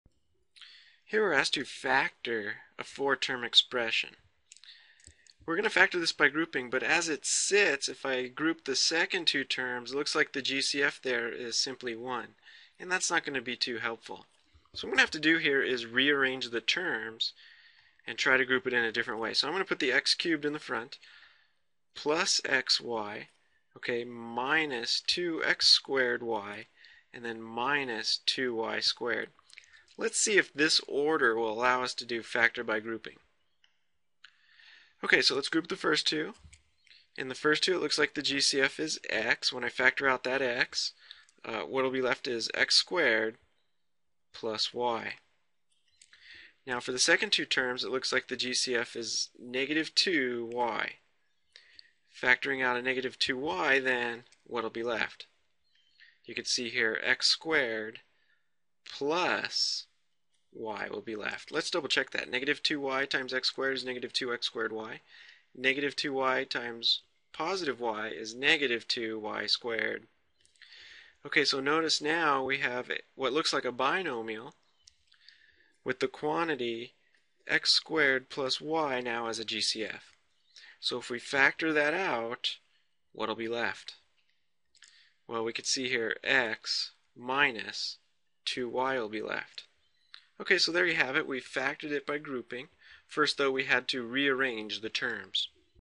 Factor by Grouping after Rearranging the Terms - A 4 term Polynomial Expression with Two Variables
Factoring 4-Term Polynomials by Grouping Steps: Now we will use the idea of factoring out the GCF in a technique called factoring by grouping of four-term polynomials.
Step 1: Group the first two terms and the last two terms.  Factor out the GCF of both groupings.
Step 2: If the remaining binomial factors are the same factor it out.
Step 3: Check by multiplying.

Steps for factoring out the GCF and factor by grouping, introduction to factoring notes